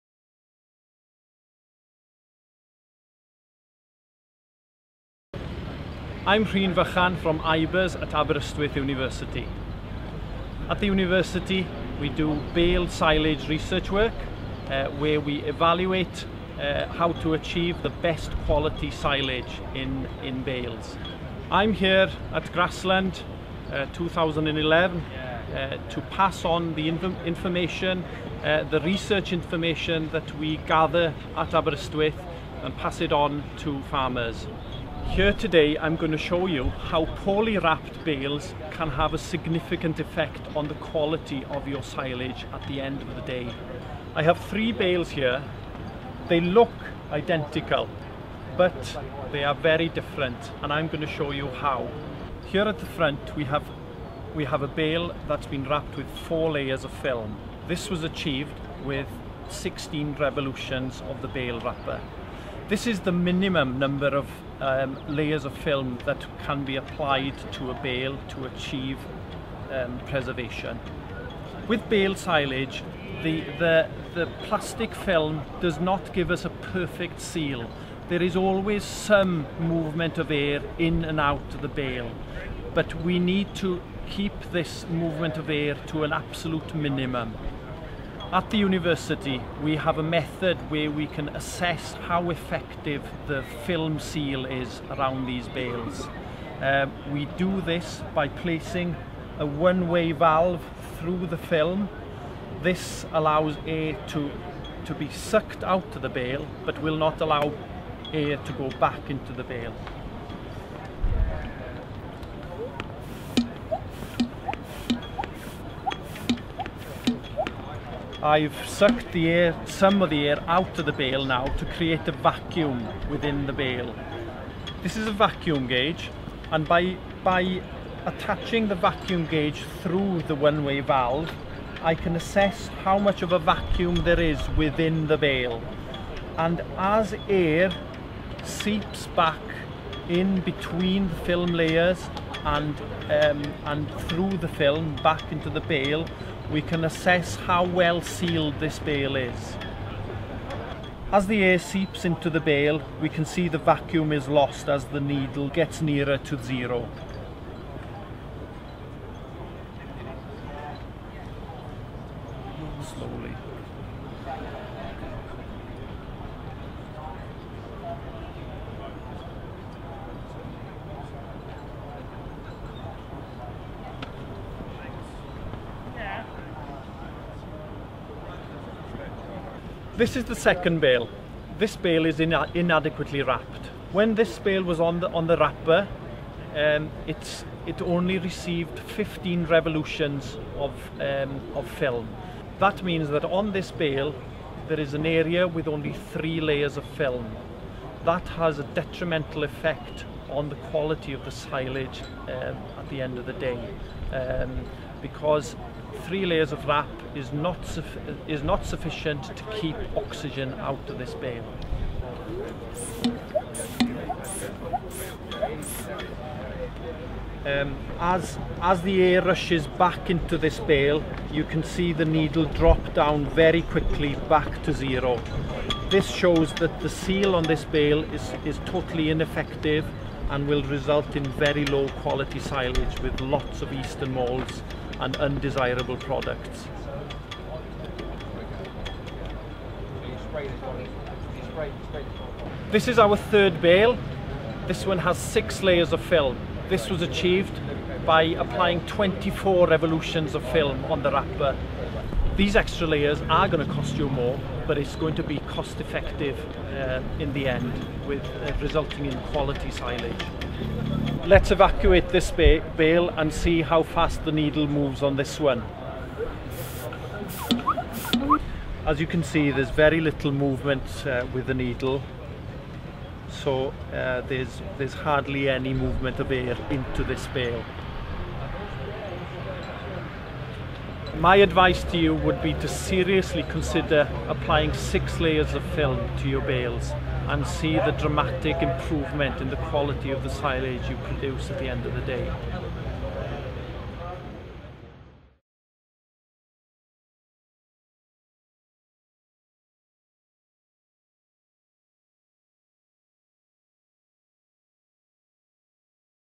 Bale Pressure Testing with Rhun Fychan
Rhun Fychan, Research Scientist at IBERS and expert for the Silage Advisory Centre (SilAC), demonstrates how the quality of silage is affected by how the bale is wrapped and shows you how this can be analysed using Pressure Testing equipment.